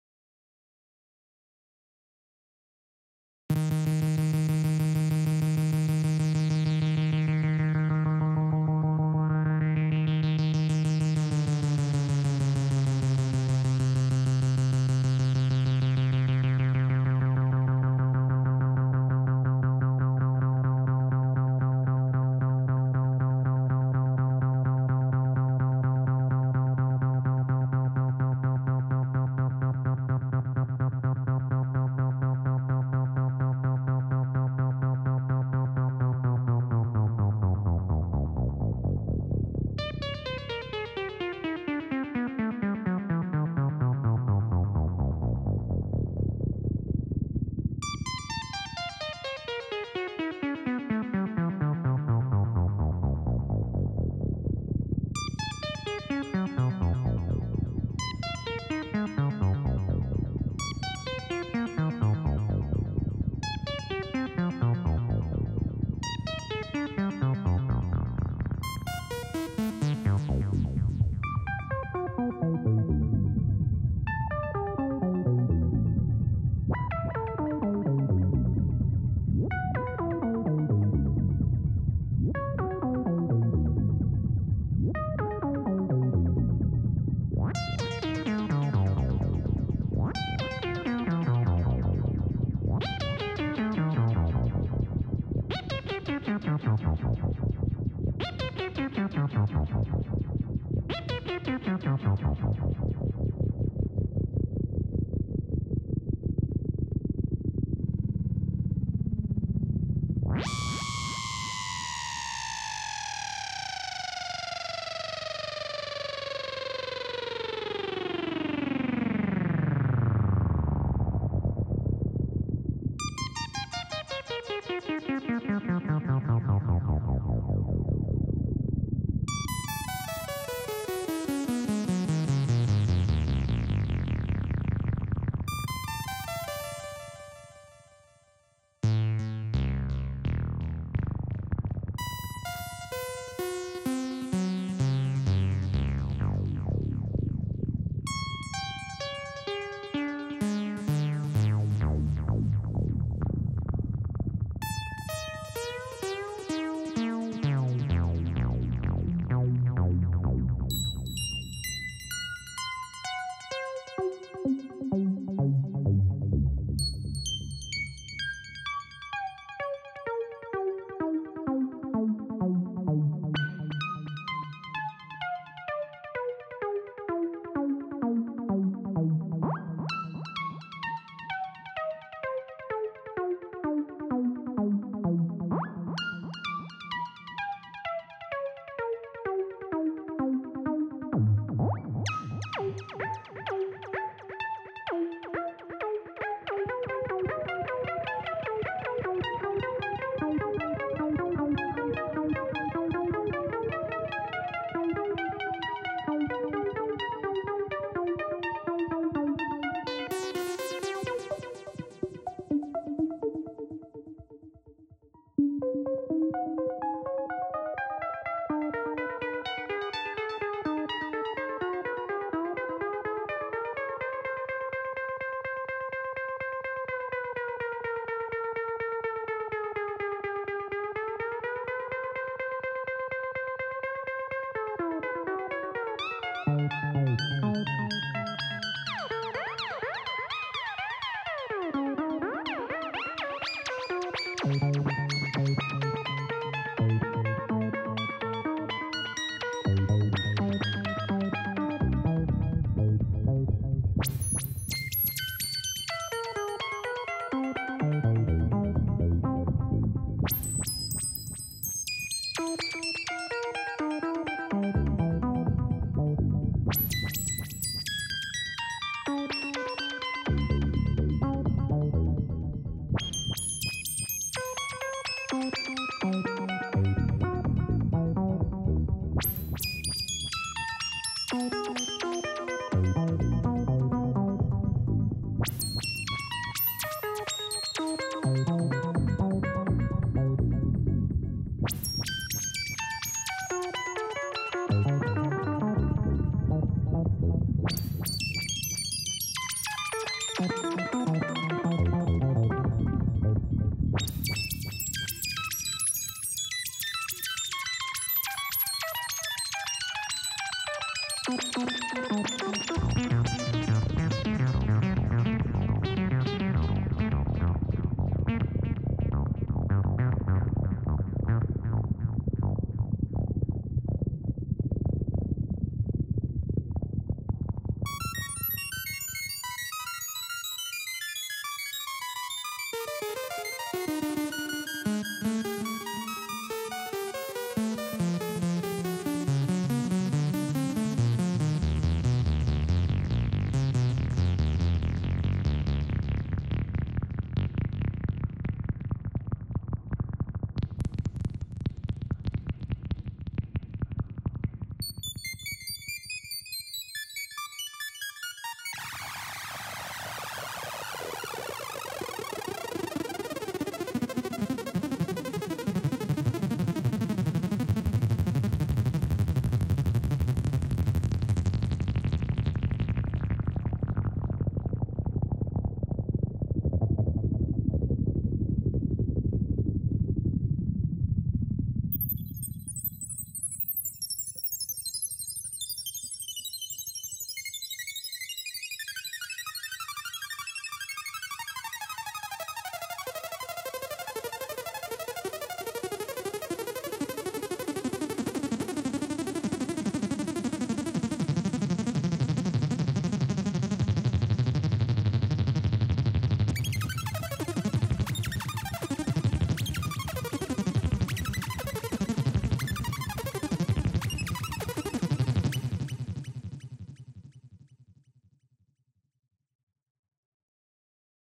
Moog 1125 Sample & Hold Controller modelled in Voltage Modular
An attempt to model the very rare Moog 1125 Sample & Hold unit.